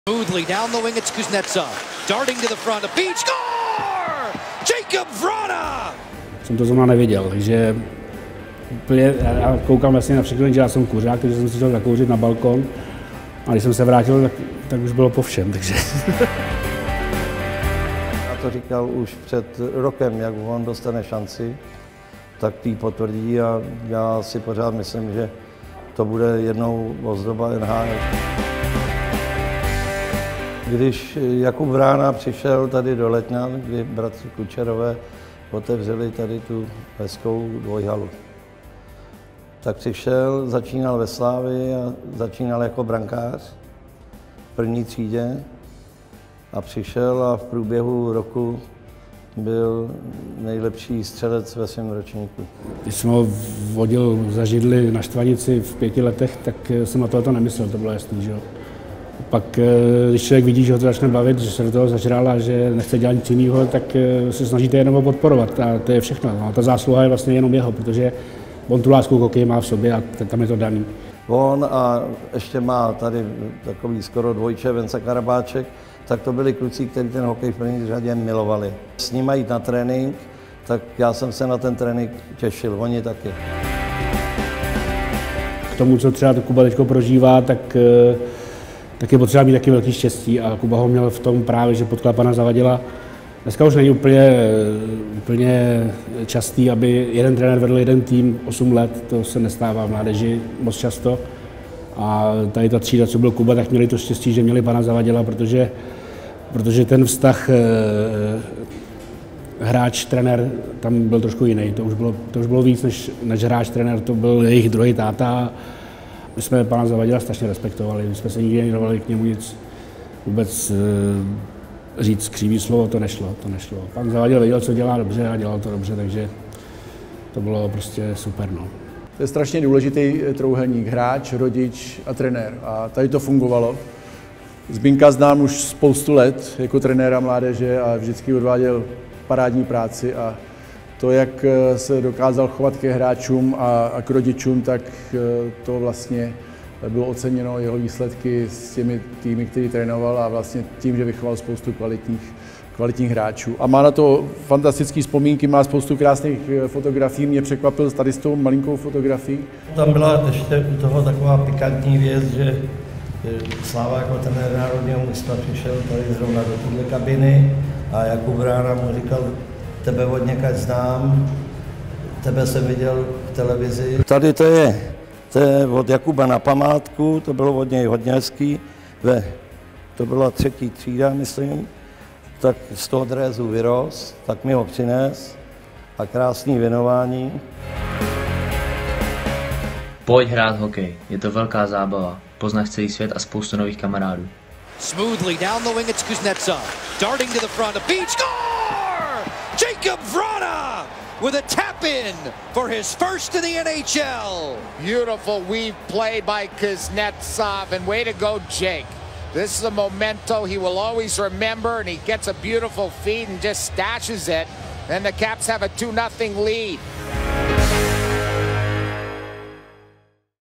Z malého klubu až do NHL. Jakub Vrána ukázal jednu z možných cest
Víte, že útočník Jakub Vrána začínal jako brankář? Není tomu tak dávno, kdy tento mladý útočník skóroval v dresu Washington Capitals. Co o Kubově prvním gólu v zámořské NHL řekl jeho tatínek? Co si o Jakubově budoucí kariéře myslí jeho bývalý trenér Letňan Zbyněk Zavadil? To a daleko víc se dozvíte v krátkém video medailonku.